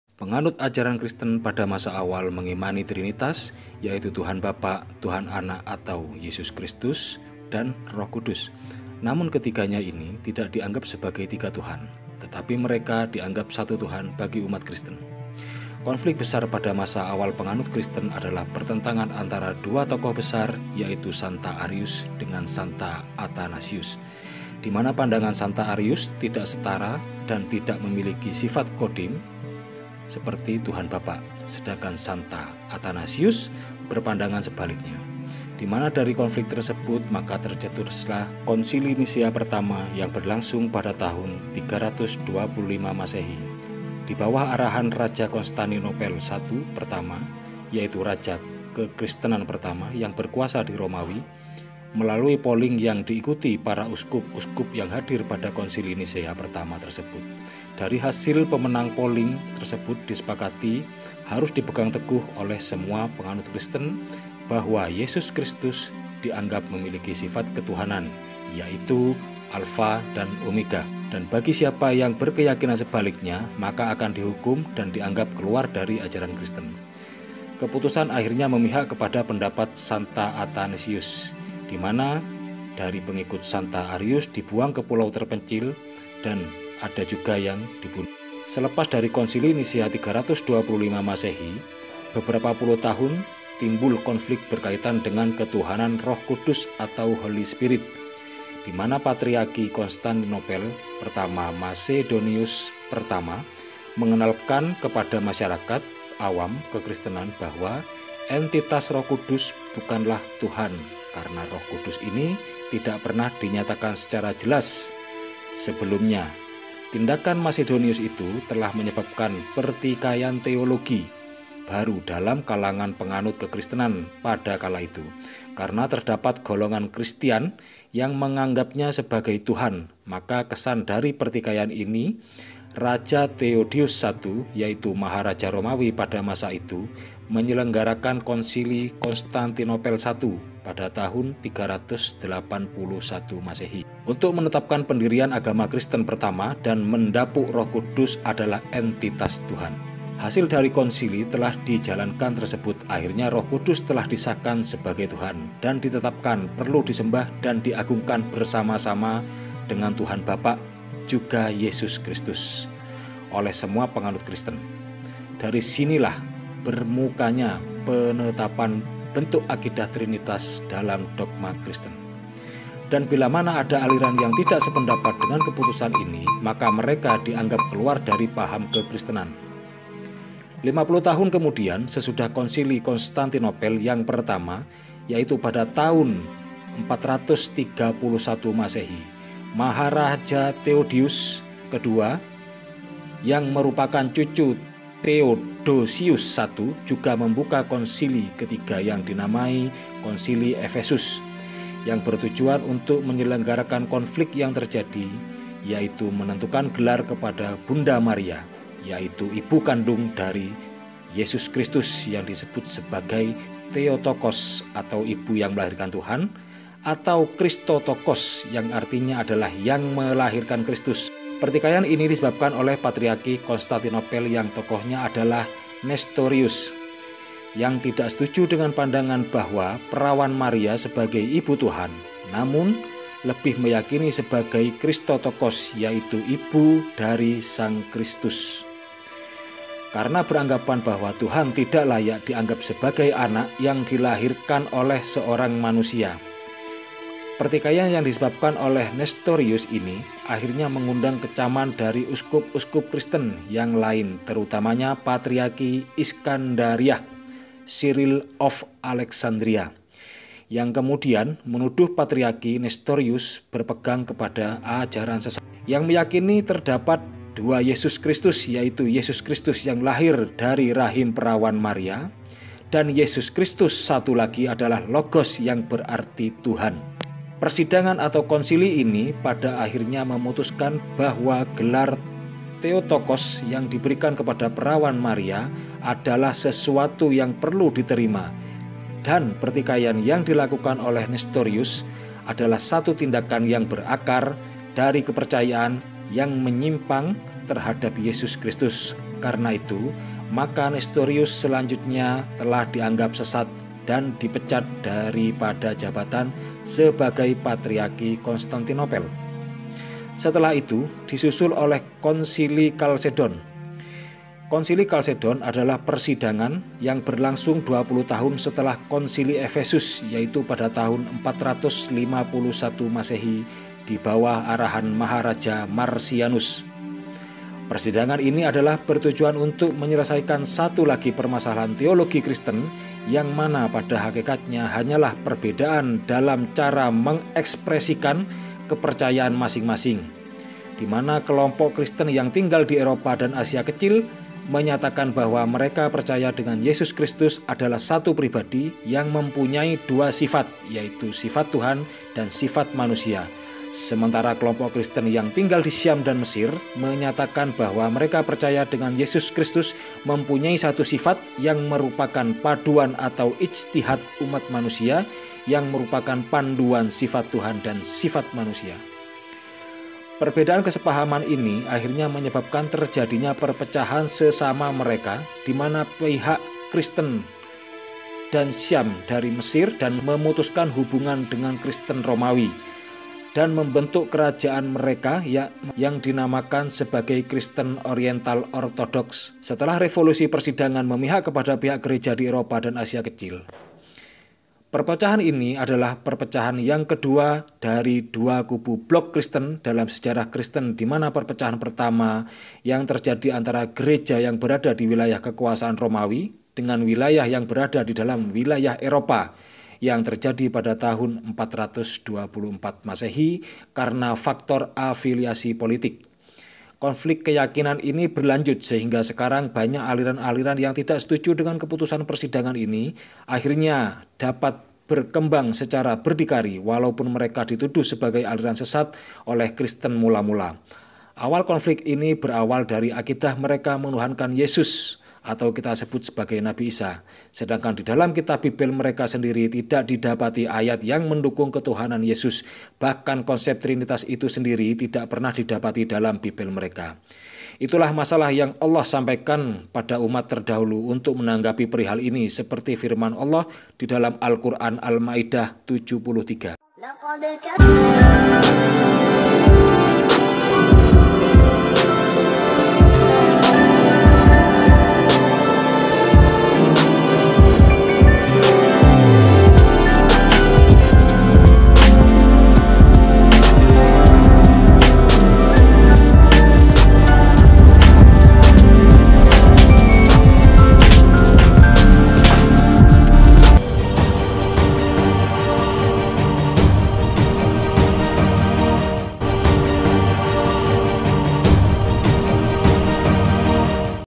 YESUS TUHAN & PERDEBATANNYA SEPANJANG MASA
YESUS MENJADI TUHAN DAN PERDEBATAN PANJANG
DIMULAI DARI BERKALI KALI DILAKUKAN DEBAT TEOLOGIA BAPA BAPA GEREJA ABAD MULA MULA SAMPAI HARI INI